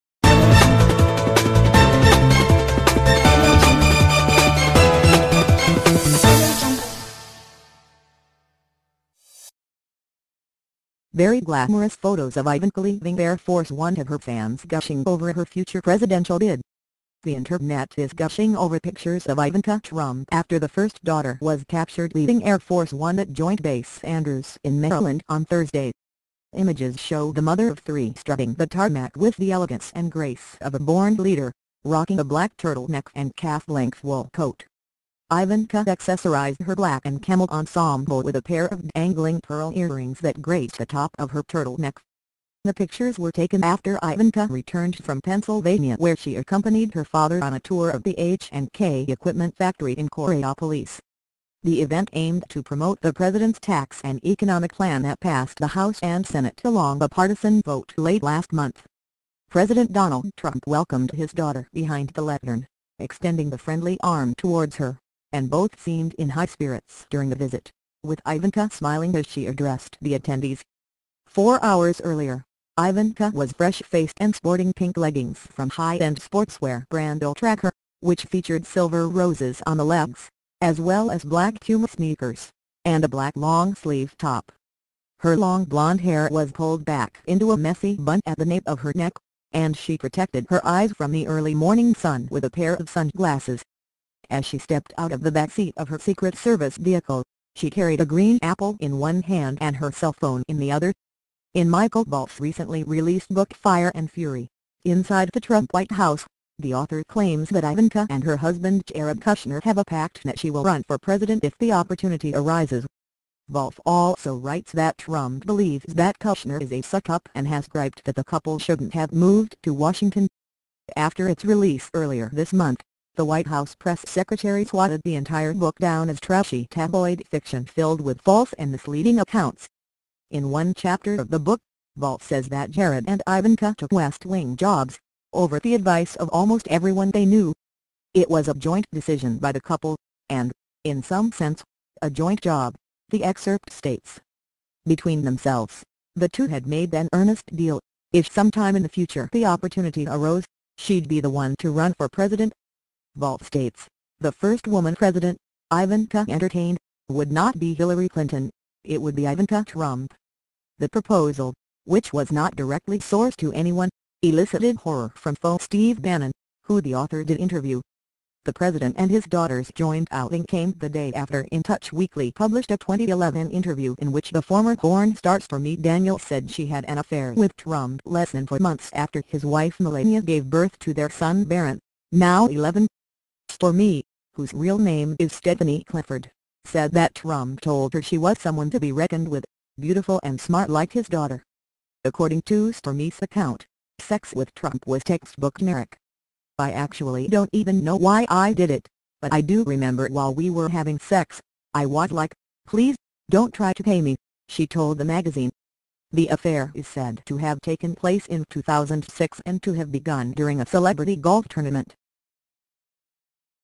Photos of Ivanka when down off the plane have fans surprise over her future presidential bid
The internet is gushing over pictures of Ivanka Trump after the First Daughter was leaving Air Force One at Joint Base Andrews in Maryland on Thursday